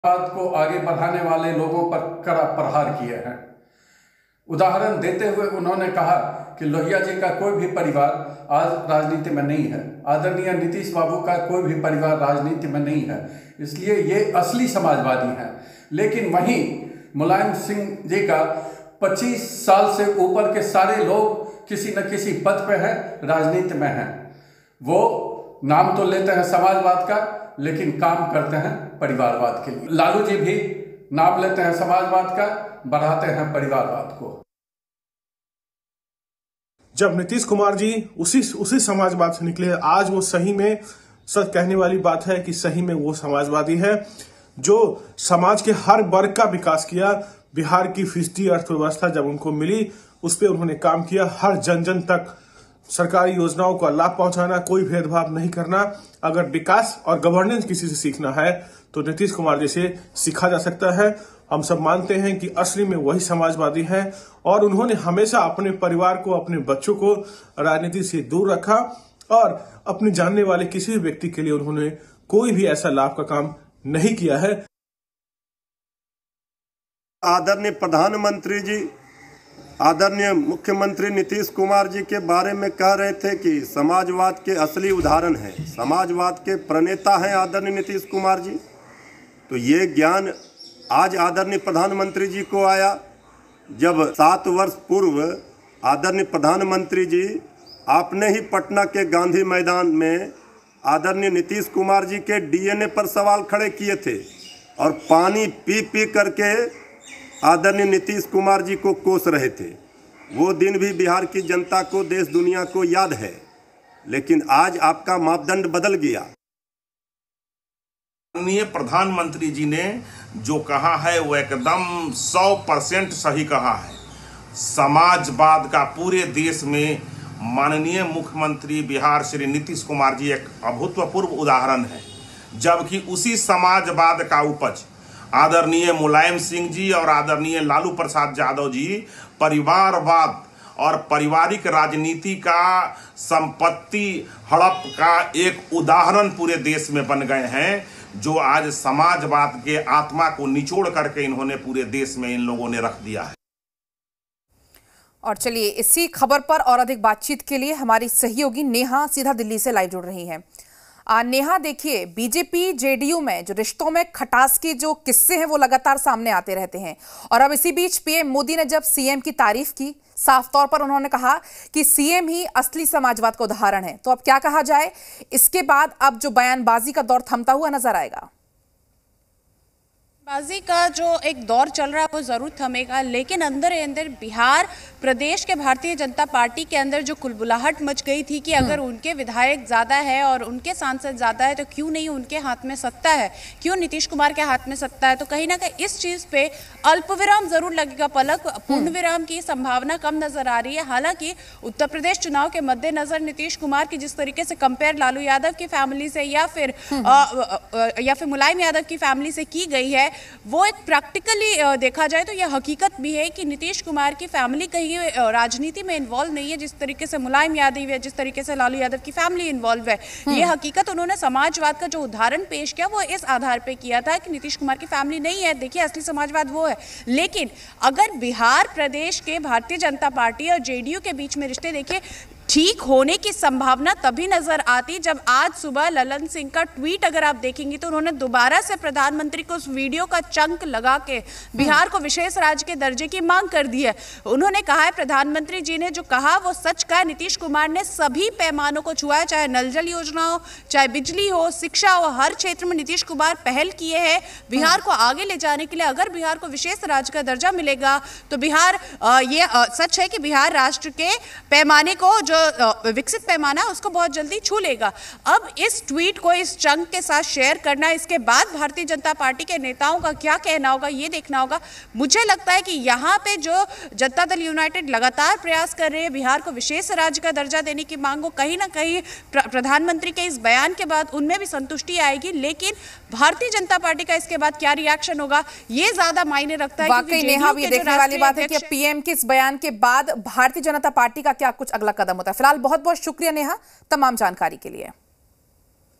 Pm Modi ने की Cm Nitish की तारीफ, कहा 'नीतीश कुमार असली समाजवादी' । Pm Modi Interview
Pm Modi praised Cm Nitish, saying 'Nitish Kumar is a real socialist'. PM Modi Interview

प्रधानमंत्री नरेंद्र मोदी ने अपने खास इंटरव्यू में बिहार के मुख्यमंत्री नीतीश कुमार की तारीफ की है. उन्होंने कहा-  लोहिया जी का परिवार कहीं नजर आता है क्या?, जॉर्ज फर्नांडिस का परिवार कहीं नजर आता है क्या?, नीतीश बाबू (PM Modi On Nitish Kumar) का परिवार कहीं नजर आता है क्या?...पीएम मोदी ने कहा कि 'नीतीश कुमार असली समाजवादी हैं'